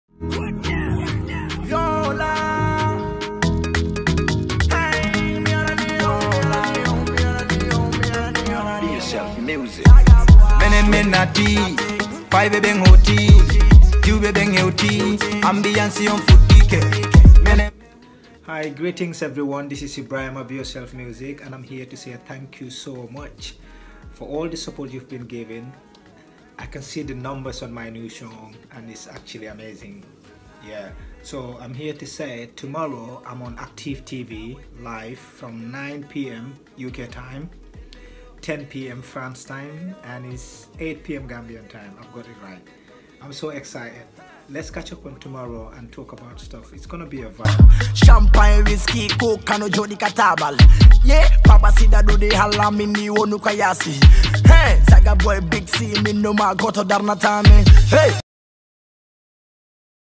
Top Gambian Artist Ebrima (Big Sey) Is Our Guest Tomorrow - The INTERVIEW
ACTIVE TV is created to bring you news from the Gambia and the other parts of The World, we are also here to bring you perfect entertainment, reviews reactions & Sport.